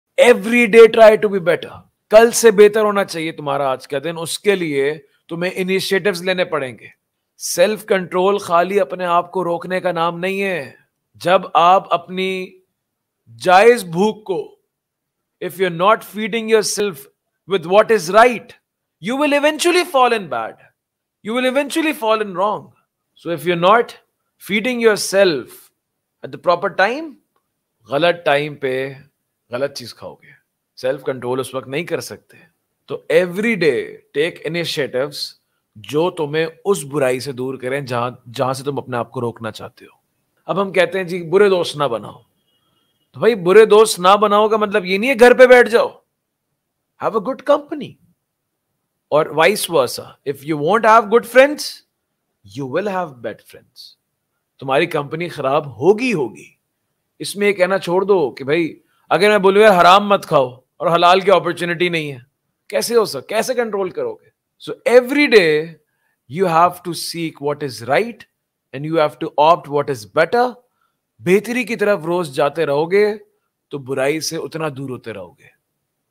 Self-Control - Burai Se Bacho | Motivational Session by Ustaad Adeel Arfeen
A motivational video for youth by Ustaad Adeel Arfeen about Self-Control & Burai Se Bachao. As Muslims, we all try and want to be free of sins, bad decisions, and anything which is prohibited in Islam but we are not strong enough to fight ourselves and our will. Obviously, the change we want in ourselves cannot be achieved overnight, it will take practice and patience. But one thing we can all do is we can try to make our day better than yesterday and we have to seek what is right and opt for what is better. This consistency is the key to self-control.